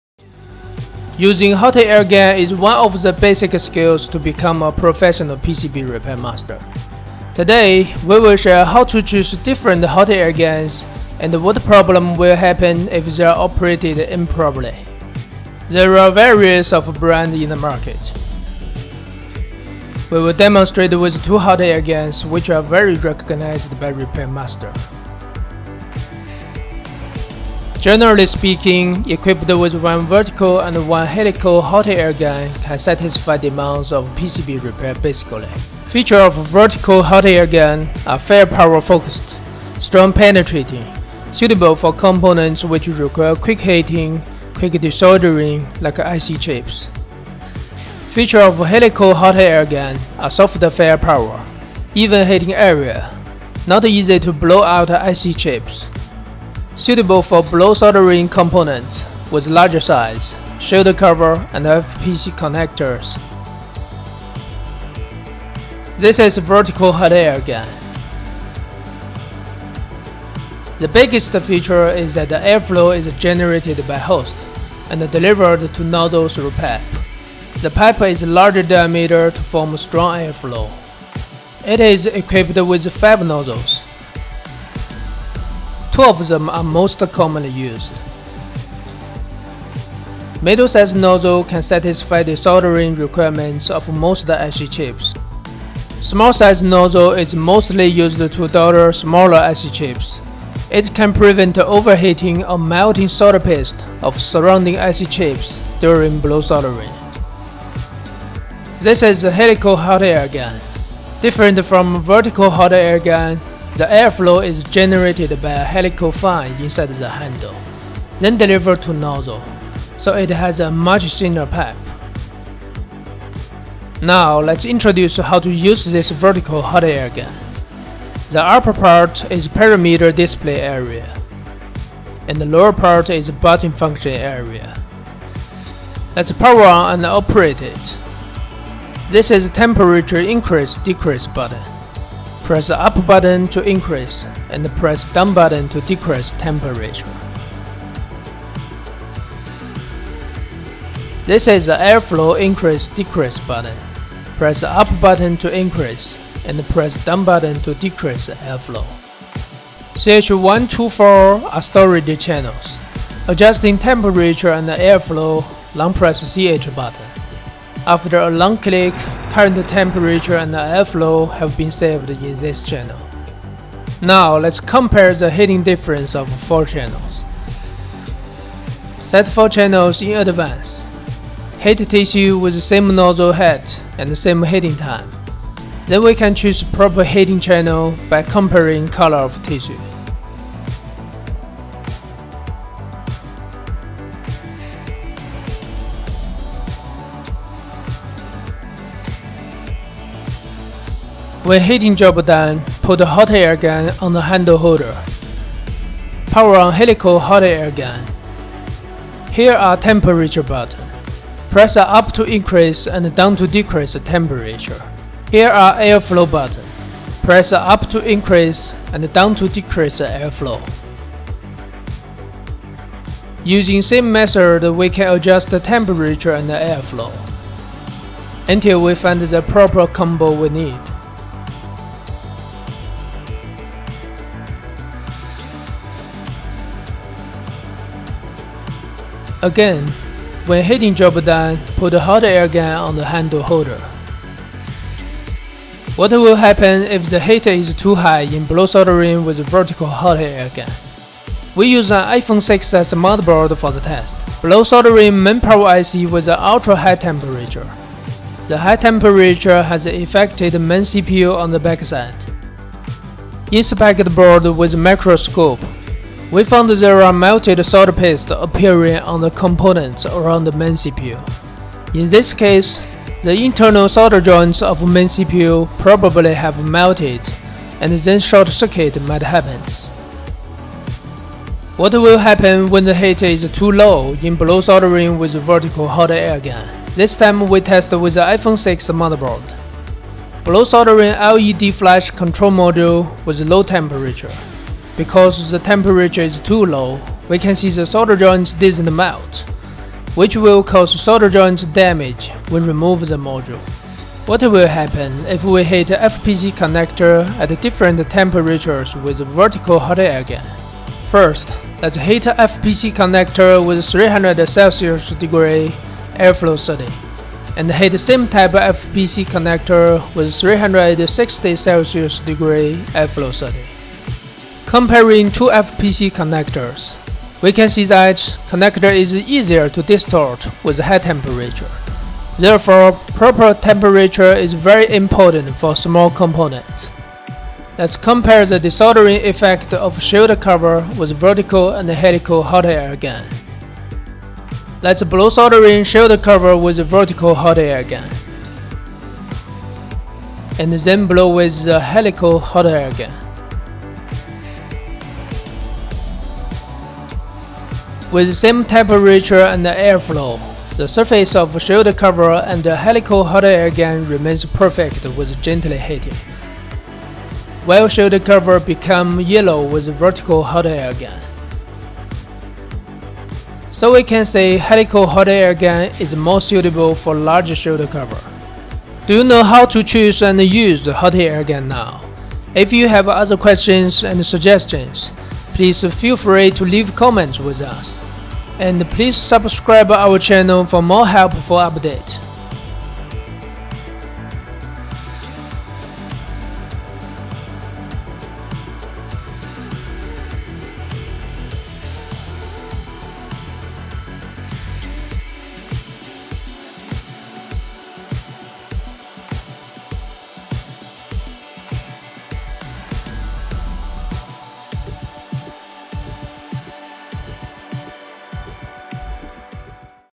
Hot Air Gun Skills
SUGON 8610DX VS SUGON 858D as an example to show how to use hot air gun

For more information or inquiry, welcome to leave a message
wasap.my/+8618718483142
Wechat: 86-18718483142